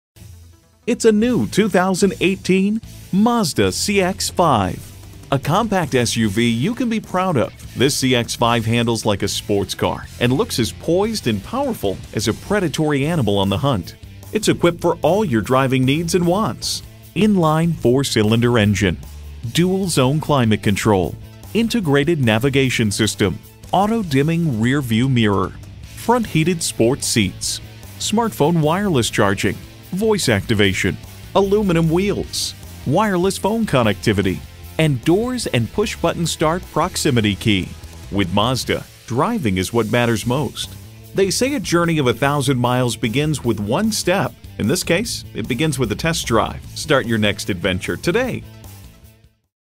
New 2018 Mazda CX-5 Baltimore, MD 5M856350 - SOLD
Year: 2018
Make: Mazda
Model: CX-5
Trim: Touring
Engine: 2.5L 4-Cylinder
Transmission: 6-Speed Automatic
Color: White
Interior: Black
Stock #: 5M856350
VIN: JM3KFBCM0J0456350

Address:
6616 Baltimore National Pike